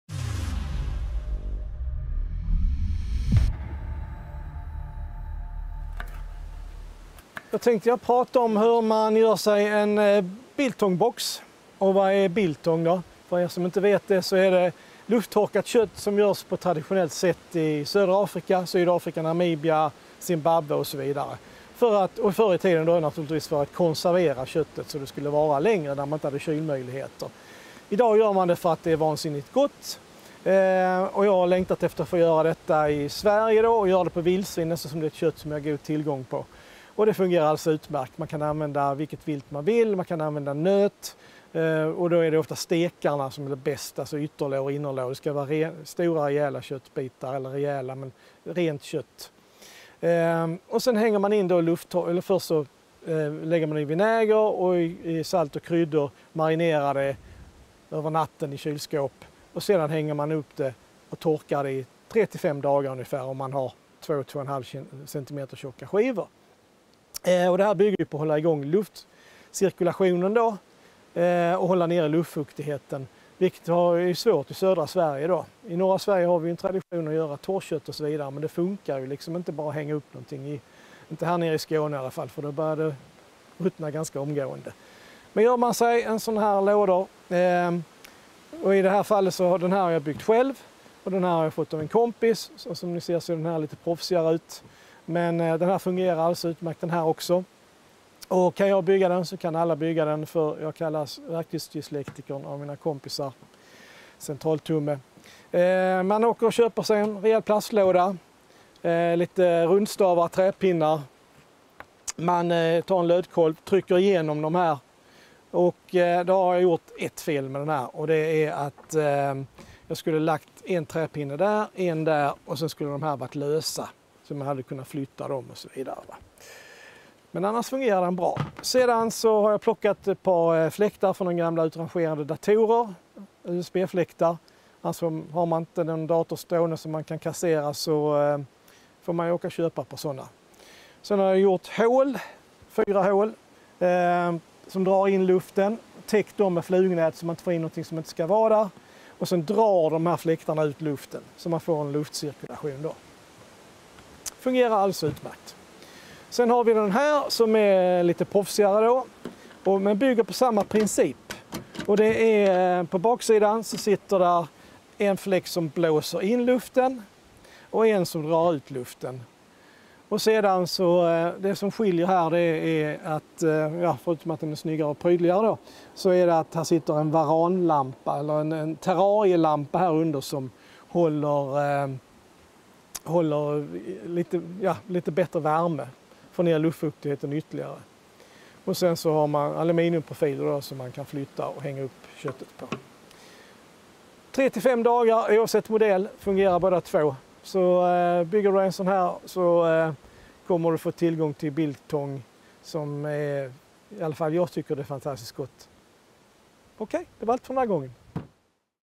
Gör din egen Biltong Box - torklåda för torkat kött
Biltong är torkat kött, gjort efter traditionellt recept från södra Afrika. I filmen visar jag hur du bygger en torklåda enkelt och billigt. Efter att marinerat köttet i vinäger och kryddor i några timmar, så torkas köttet i 3-5 dagar. Sedan har du en fantastiskt god biltong, perfekt som färdkost eller tilltugg till en kall...